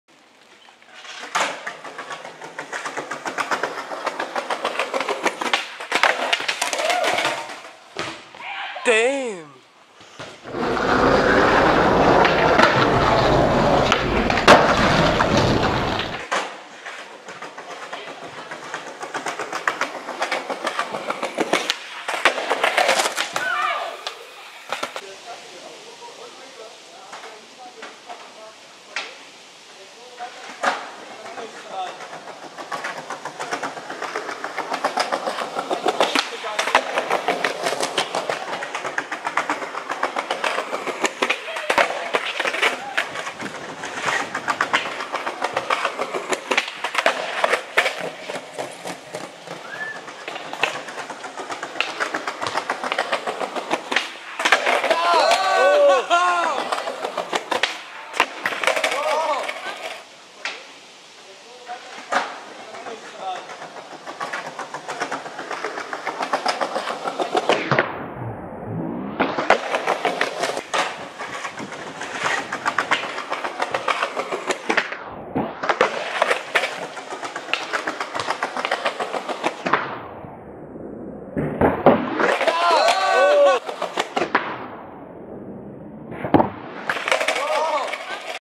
DTS Sven some tricks...
chill footage..destroy the streets
kickflip 4 stair
kickflip
nollie bigspin 4 stair
nollie 360 shove it 4 stair